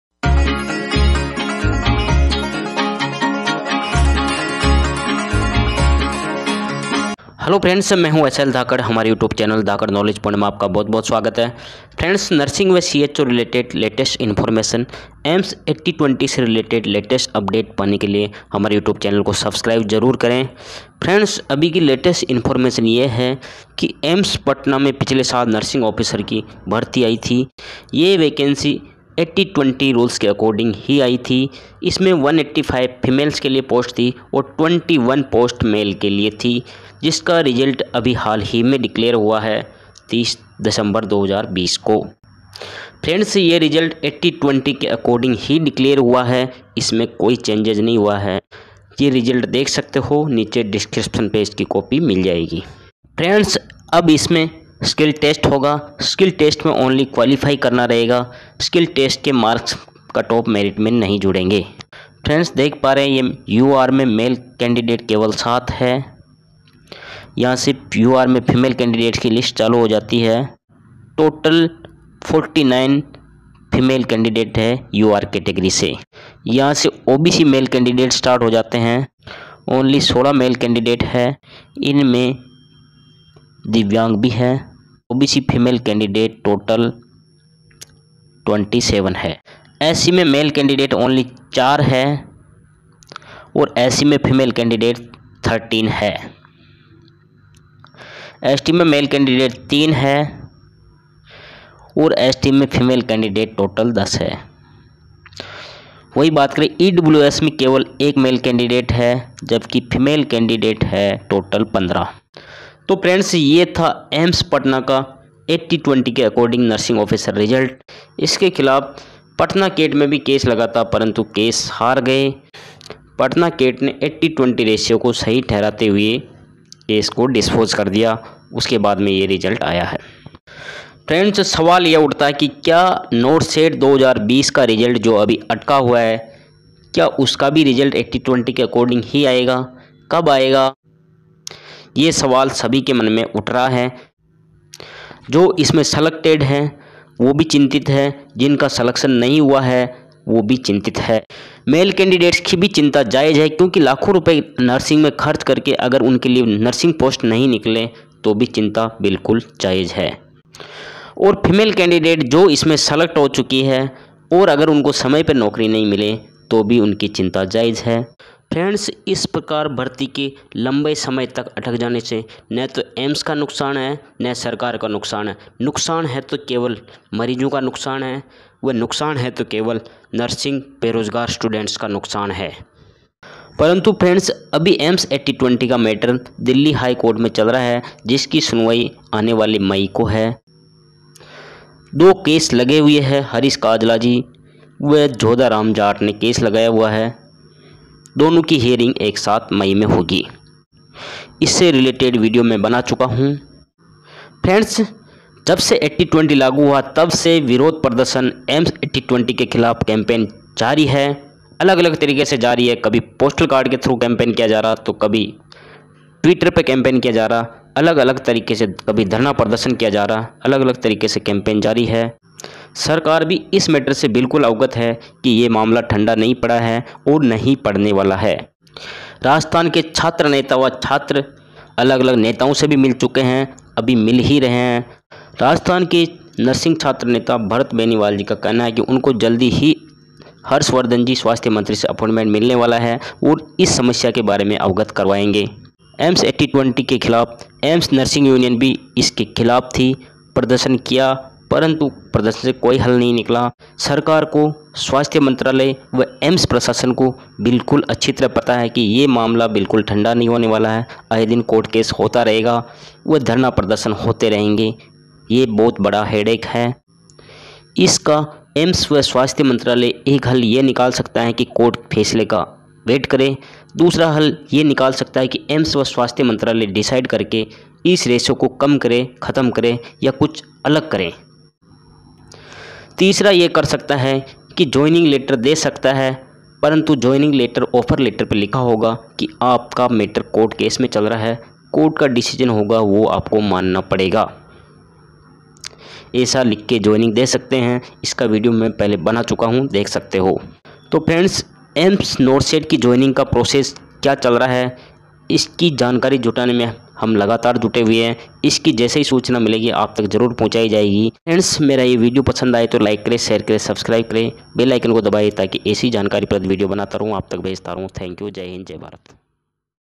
AIIMS PATNA NURSING OFFICER RESULT DECLARED ACCORDING 80:20 परंतु NORCET JOINING का क्या होगा?
aiims80:20,
AIIMS PATNA RESULT LIST-